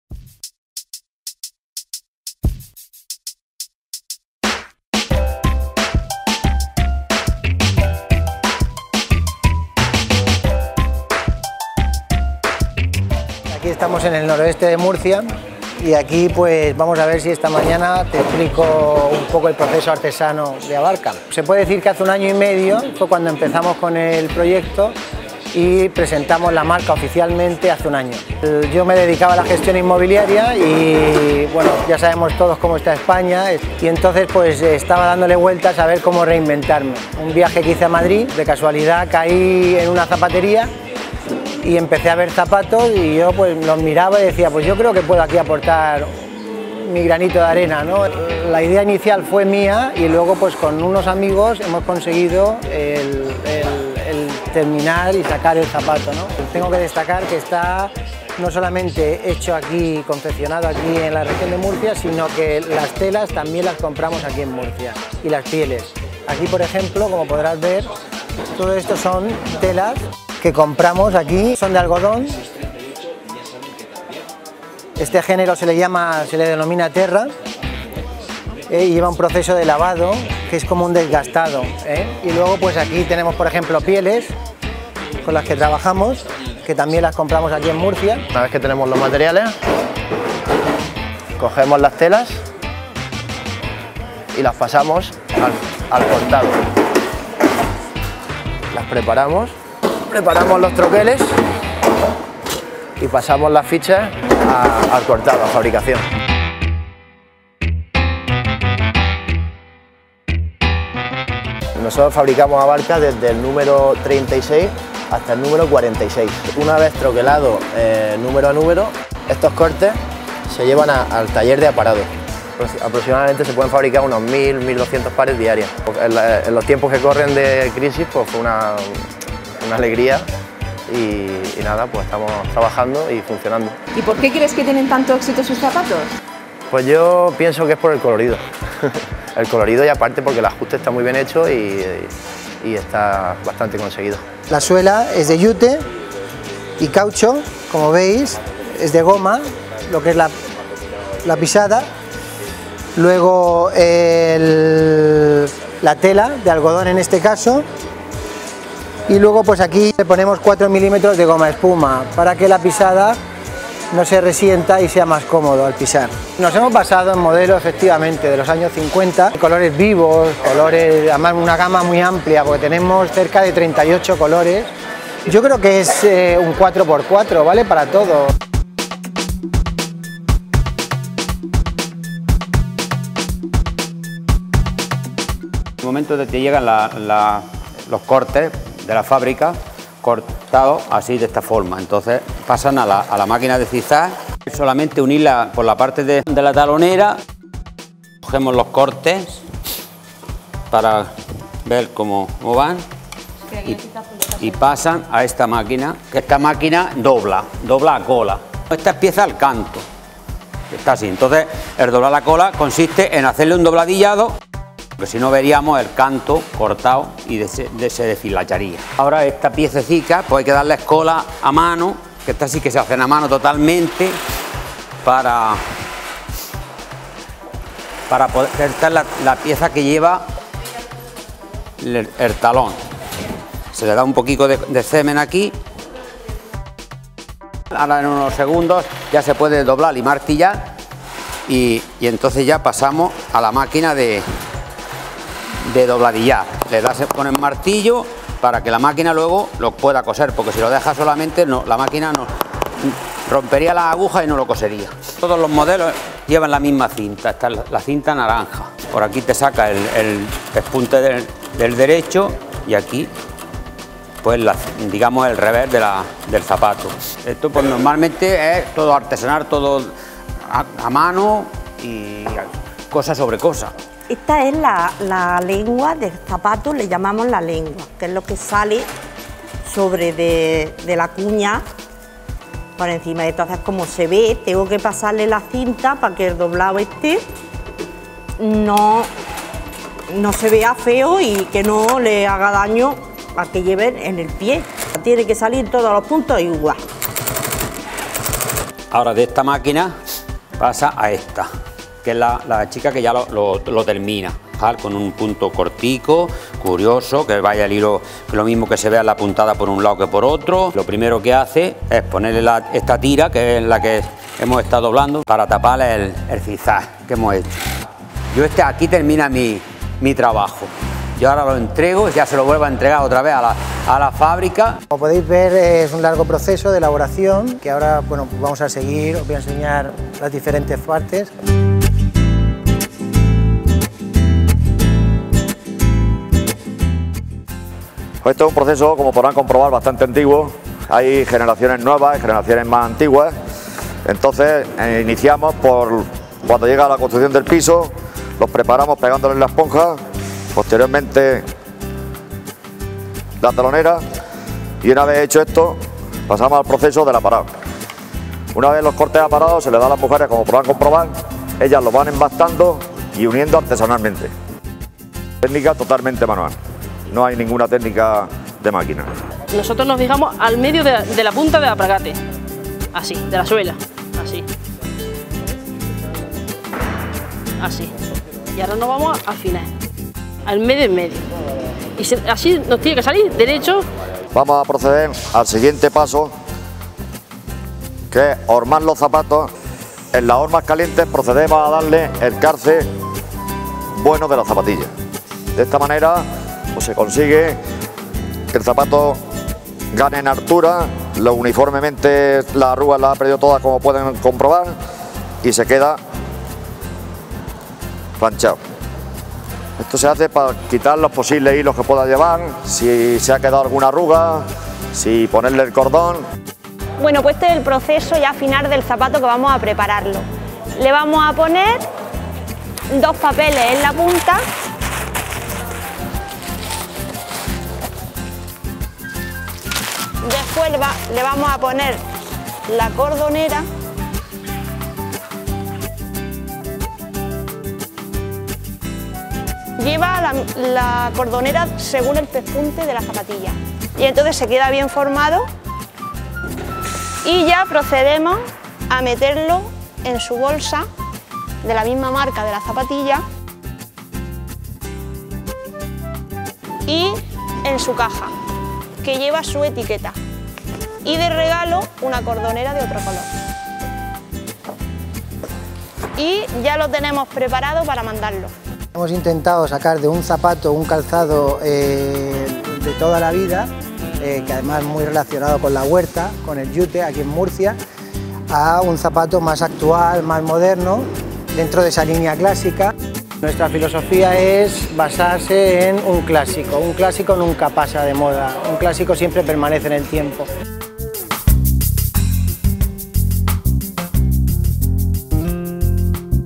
Zapatos made in Murcia
Con tan sólo un año y medio de vida la empresa de zapatos Abarca Shoes ha conseguido conquistar al mercado español, al portugués, francés e italiano. Un calzado elaborado desde principio a fin por artesanos murcianos. Unos zapatos todo terreno fabricados con yute y piel. Nos colamos en la fábrica donde miman cada una de sus piezas.